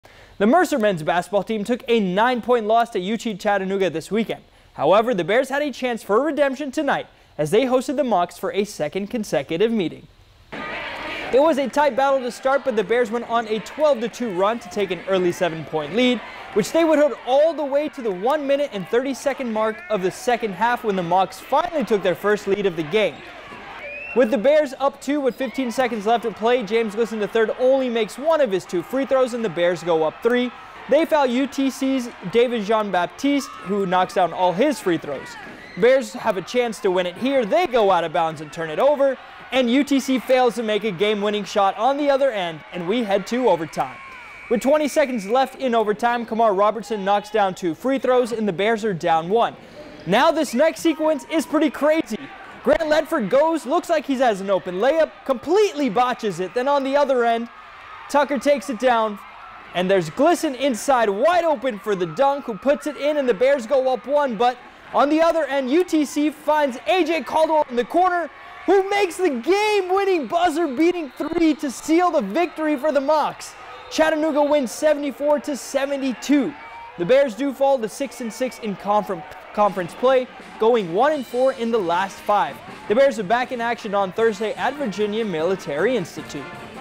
Mercer men's basketball falls in an overtime thriller
MACON, Georgia (41NBC/WMGT) --This past weekend, the Mercer men's basketball team took a nine-point loss to UT Chattanooga, but the Bears had a shot at redemption last night facing the Mocs in back-to-back games.